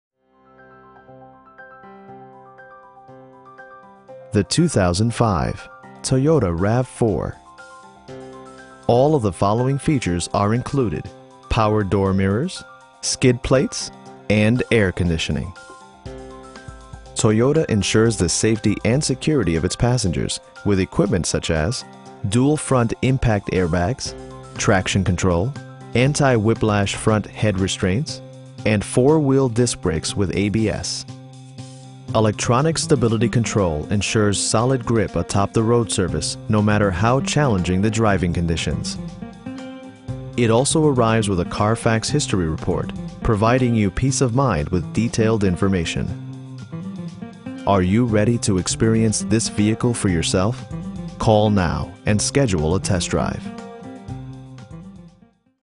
2005 Toyota RAV4 in Aurora, CO 80012
Stevinson Toyota Scion East
444 S Havana St.

The 2005 Toyota RAV4. All of the following features are included: front bucket seats, tilt steering wheel, power door mirrors, skid plates, power windows, an overhead console, and air conditioning. Toyota ensures the safety and security of its passengers with equipment such as: dual front impact airbags, traction control, anti-whiplash front head restraints, and 4 wheel disc brakes with ABS. Electronic stability control ensures solid grip atop the road surface, no matter how challenging the driving conditions. It also arrives with a Carfax history report, providing you peace of mind with detailed information. Are you ready to experience this vehicle for yourself?